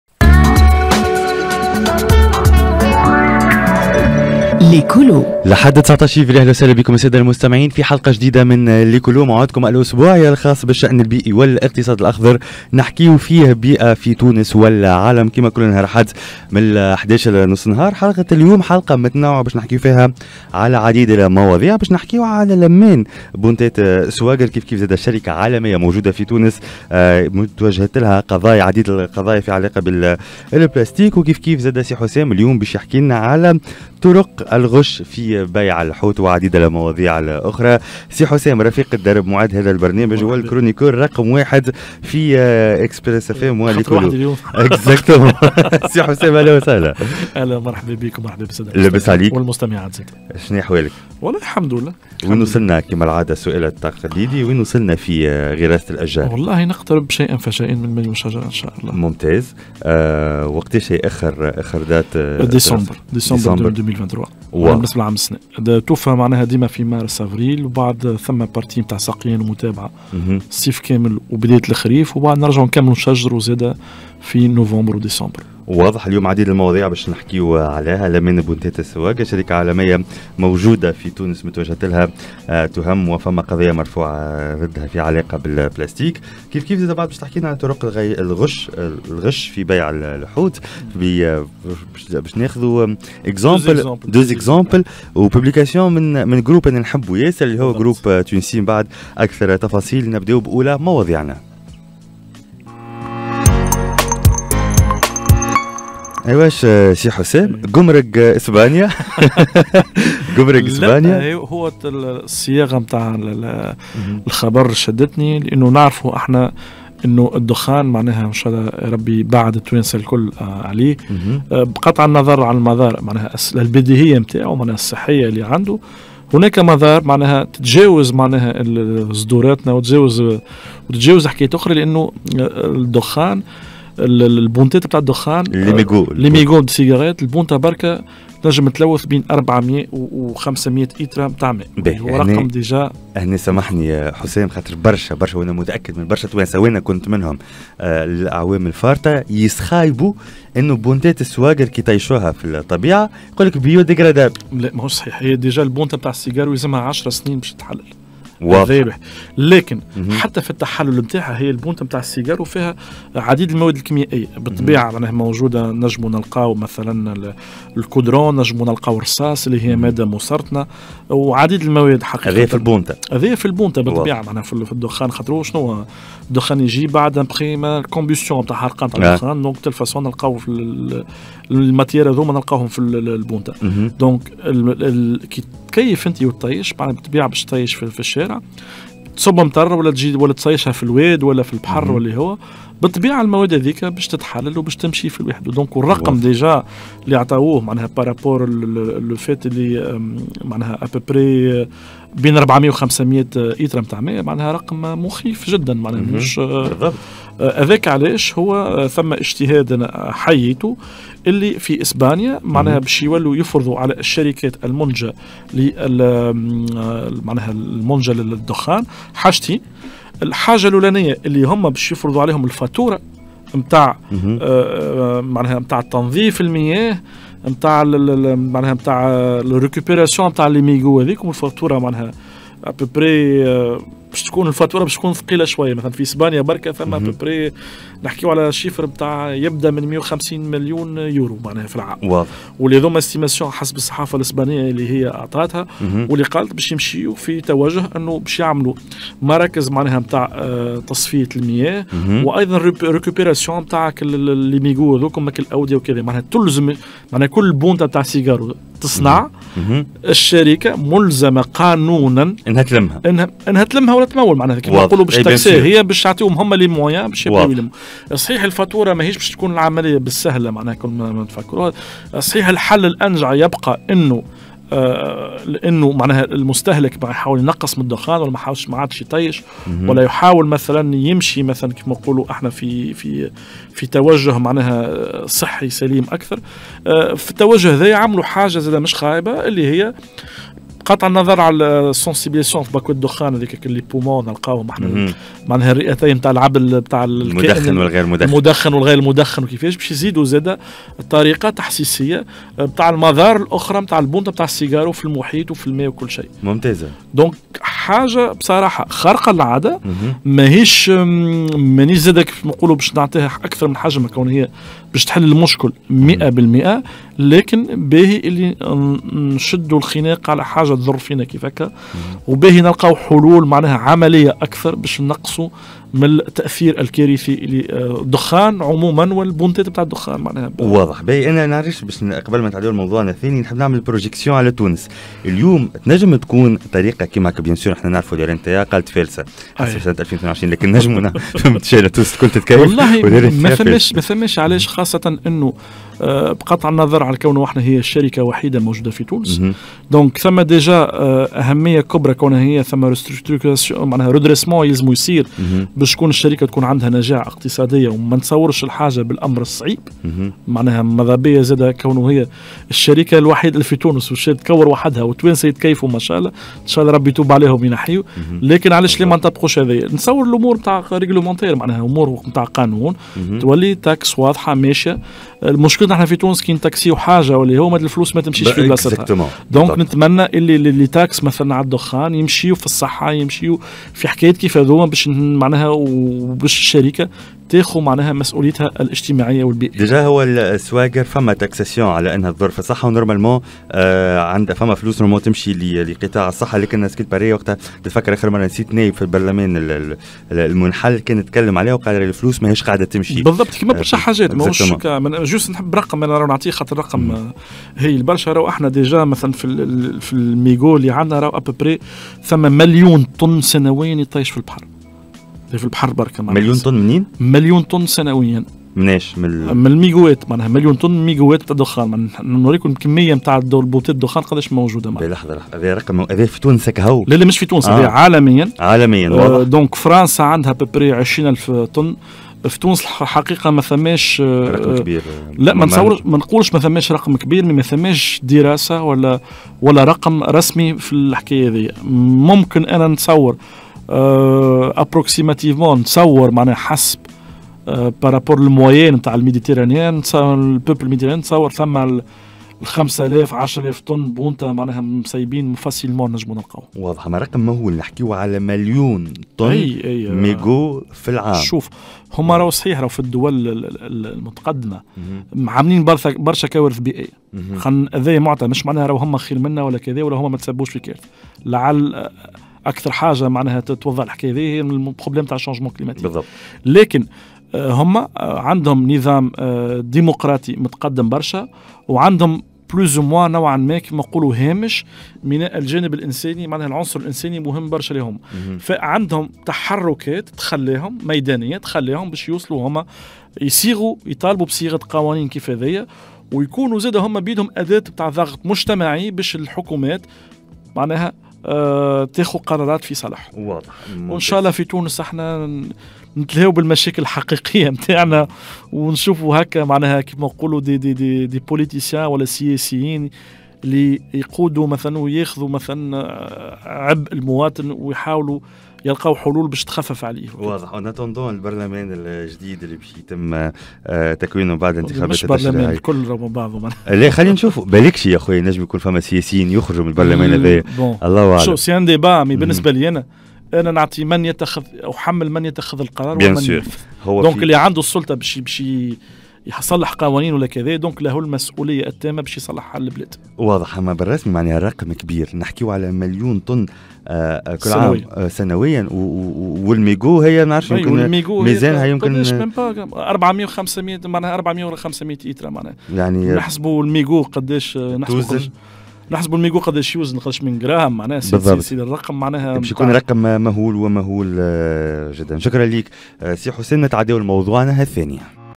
‎بونتات السواقر ماهمش Biodegradable !!
شركة  غذائية عالمية موجودق في تونس قاتت الحد في استعمال البلاستيك
طرق الغش في بيع الحوت !